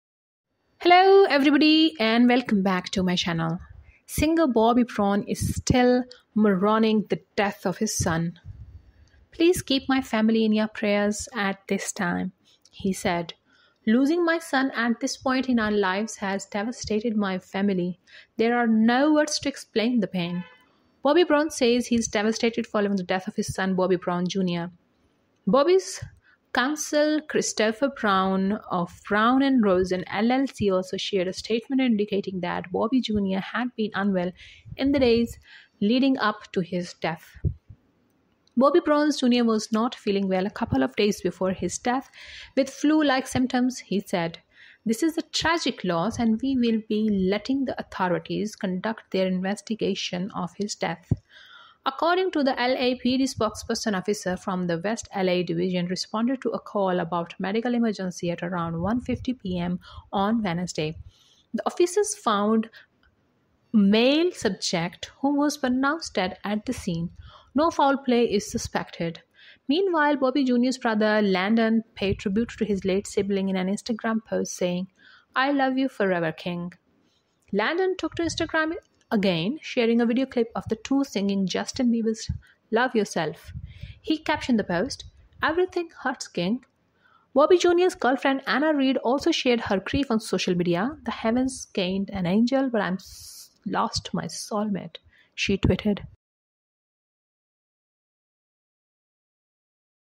Sad News For Bobby Brown. He Is Heartbroken And Revealed
Robert Barisford Brown born February 5, 1969 is an American singer, songwriter and dancer. Brown, alongside frequent collaborator Teddy Riley, is noted as one of the pioneers of new jack swing: a fusion of hip hop and R&B. Brown started his career in the R&B and pop group New Edition, from its inception in 1978 until his exit from the group in December 1985. Brown also appeared in the films A Thin Line Between Love and Hate and Two Can Play That Game. He returned to New Edition for a reunion album and tour from 1996 to 1997, and returned with all six members for another stint in 2005. Brown and Houston starred in the 2005 reality show Being Bobby Brown; Houston filed for divorce two years later in 2007. Houston died in 2012, followed by their daughter in 2015.